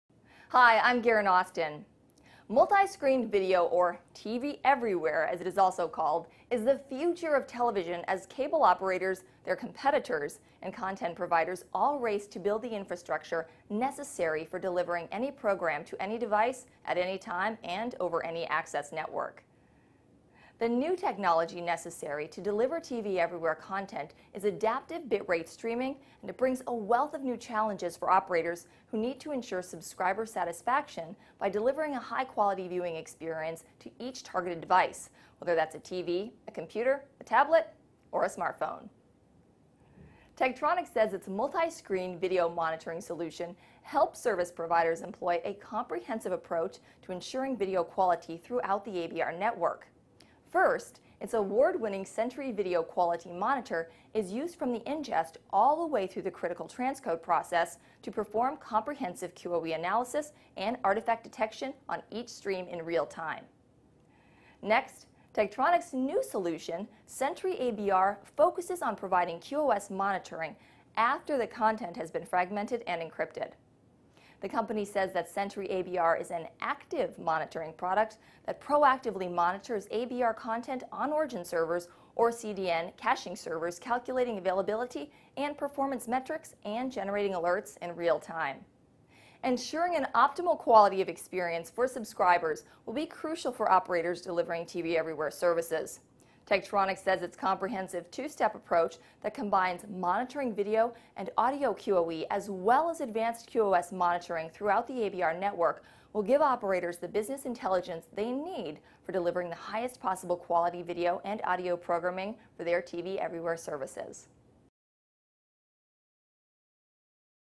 Monitoring and Measuring Adaptive Bit Rate (ABR) streams
Ensuring the quality of multiscreen video -- and the adaptive bit rate streaming technology on which it is built -- is an important and complex undertaking that requires a well thought out game plan, precise executive and proper monitoring techniques and tools.

Tektronix Sentry ABR is an "active" monitoring product that proactively monitors ABR content on origin servers or CDN caching servers.  It does this by actively requesting and validating program playlists / manifests that it has been configured to monitor.  It then requests from the server, in turn, all of the fragments of each profile / representation for each program -- calculating availability and performance metrics and generating alerts in real time.